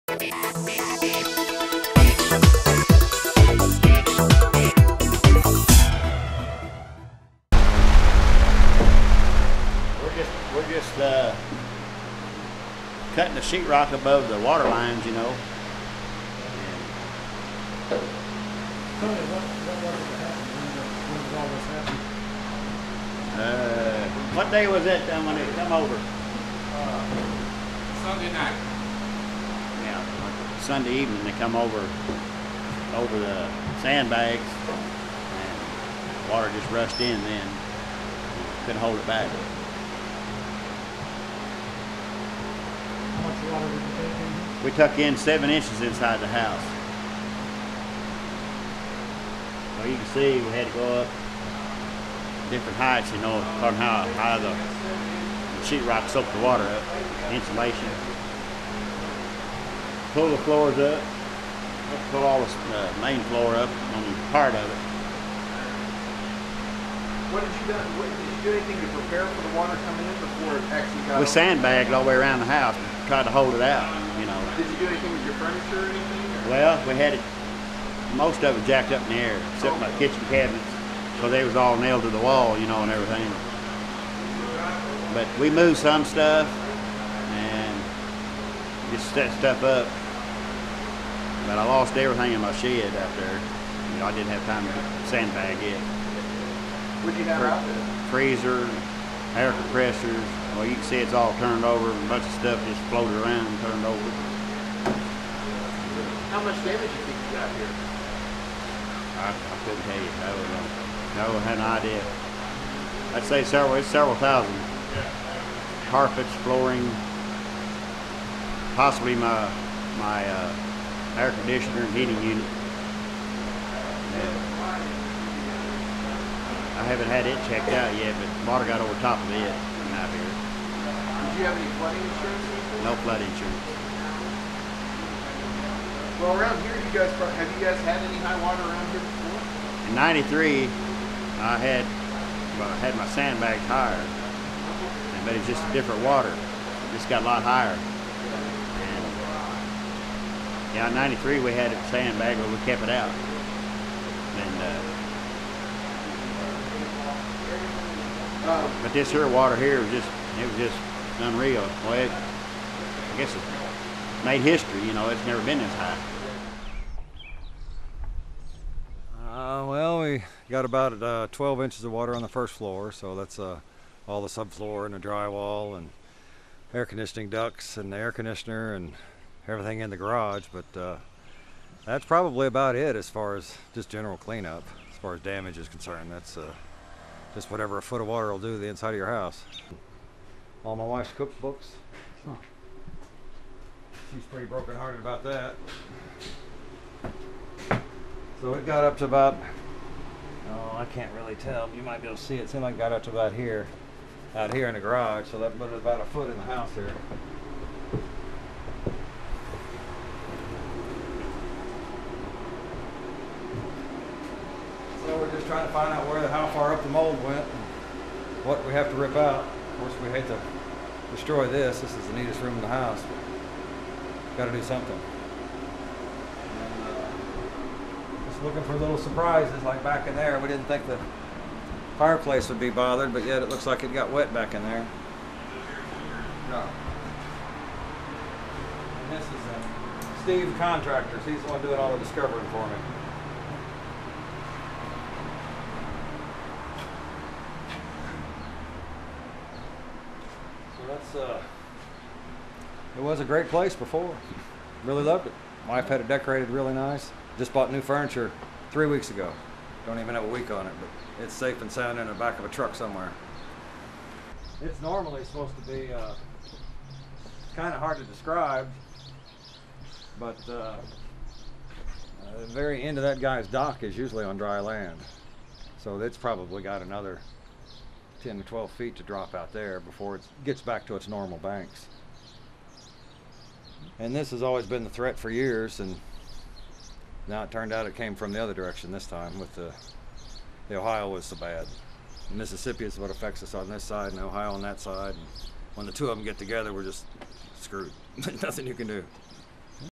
Flood Cleanup in Olive Branch
Residents in the Alexander County community of Olive Branch - one of the worst hit in the recent flood - return home and begin to clean up as the waters recede.